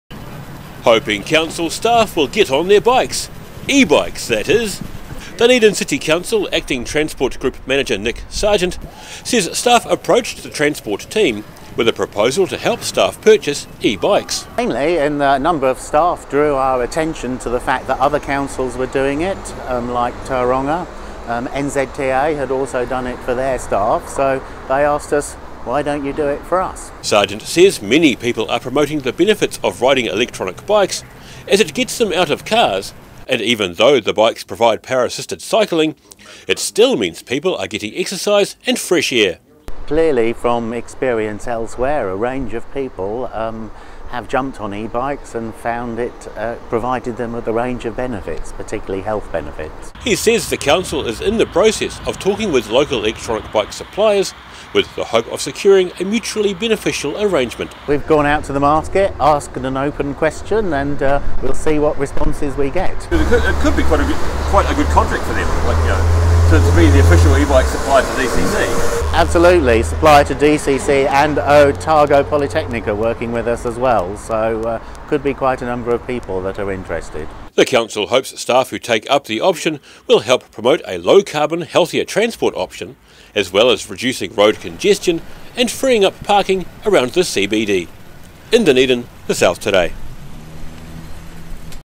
DCC looking for an E-bike supplier
The Dunedin City Council is hoping to get up to 300 of its staff back in the saddle in a proposal to give workers pay advances to buy e-bikes.